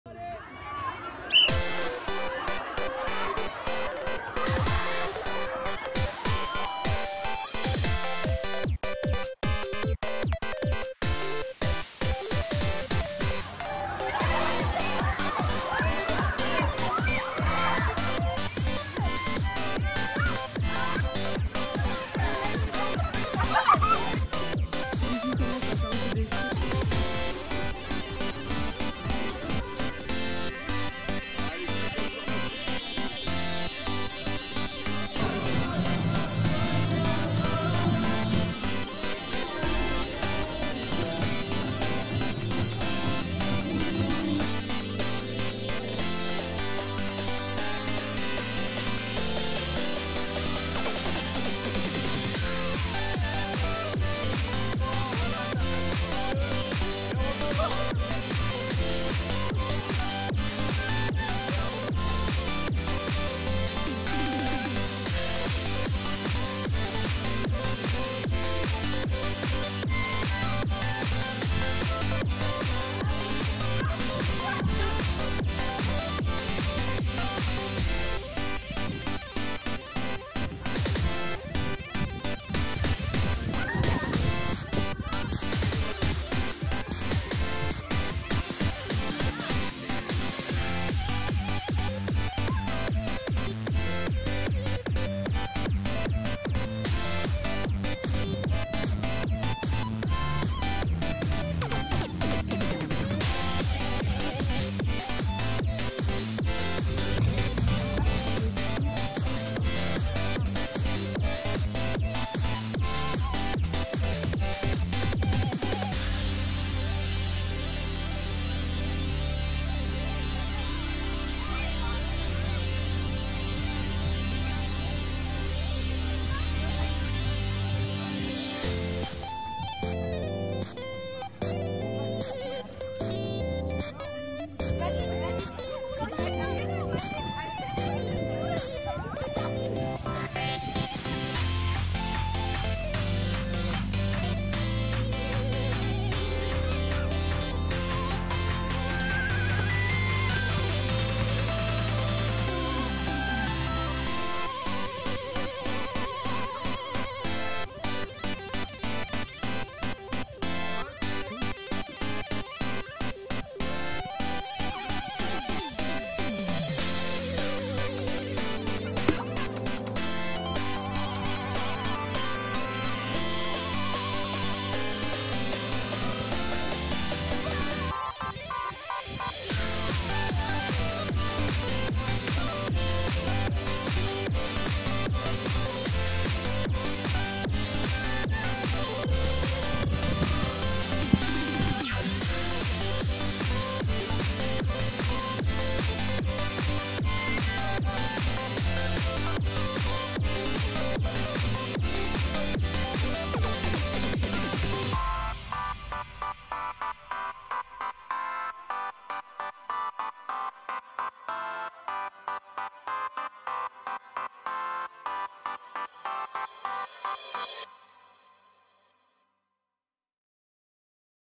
La Col-lycéenne - journée sportive UNSS 100% filles - 30 mars 2023
Tournage : Jules,
Montage : Sensei07

Réalisé par le club media du collège les Perrières le 30 mars 2023 pour l'UNSS Ardèche.

Retrouvez toutes les réalisations VR et les reportages du collège sur le site du FABLAB